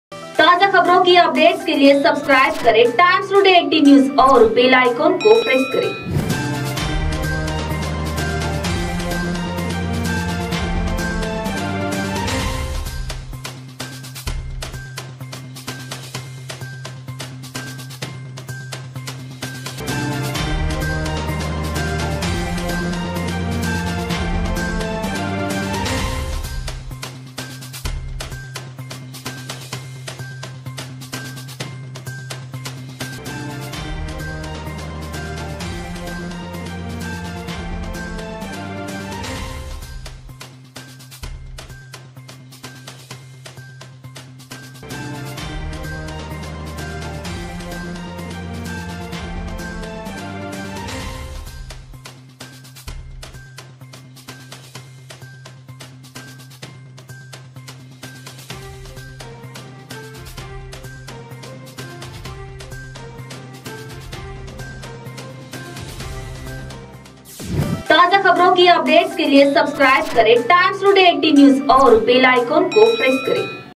SANGLI | अनेक वाहन घसरून रस्त्यावर अपघात | मनपा कडून दुर्लक्ष, नागरिक संतप्त | TIMES TODAY 18 NEWS
IMPORTANT NEWS 👇🏻👇🏻👇🏻